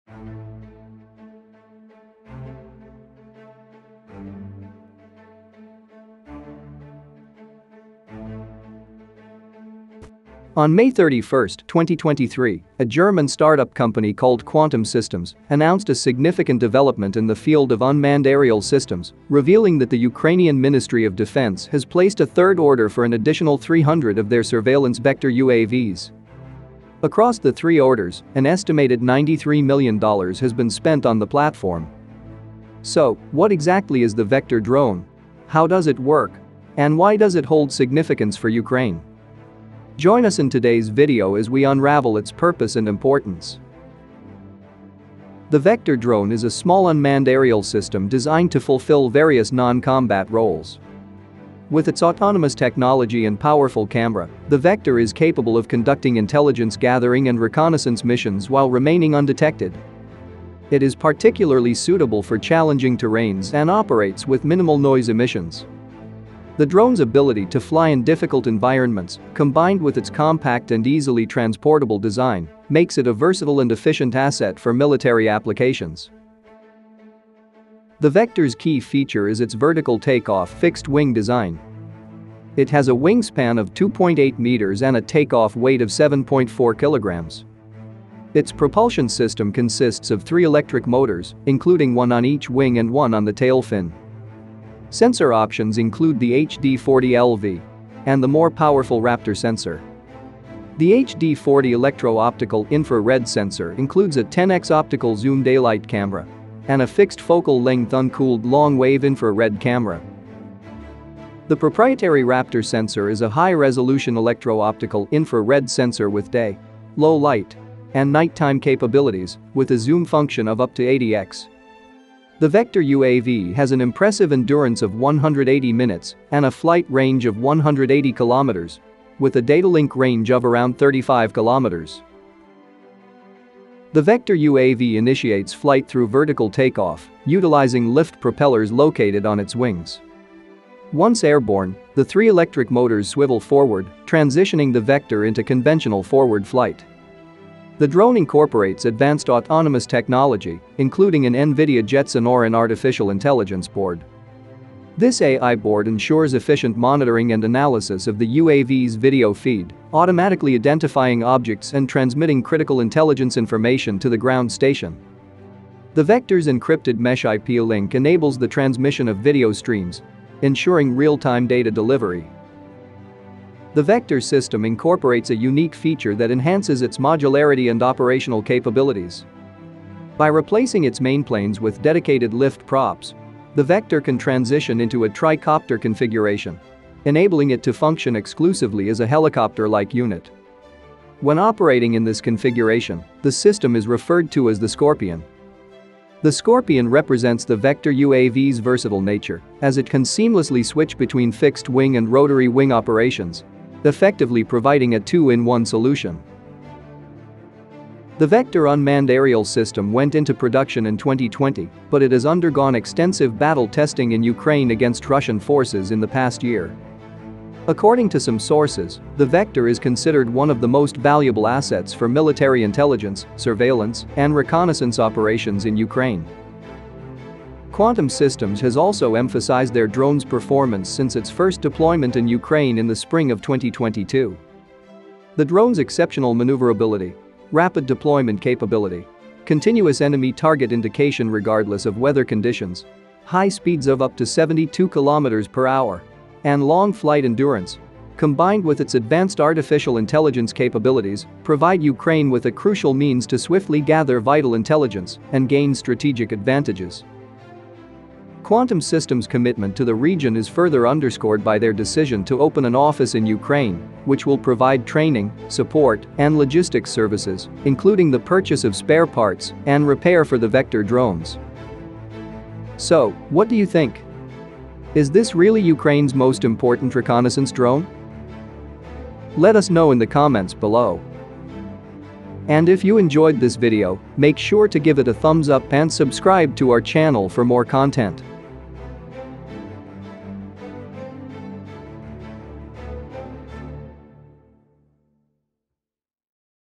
The Vector Drone: Ukraine's Most Valuable Reconnaissance Asset?
On May 31st, 2023, a German startup company called Quantum Systems announced a significant development in the field of unmanned aerial systems, revealing that the Ukrainian Ministry of Defense has placed a third order for an additional 300 of their surveillance Vector UAVs.

Across the three orders, an estimated $93 million has been spent on the platform. So, what exactly is the Vector drone, how does it work and why does it hold significance for Ukraine?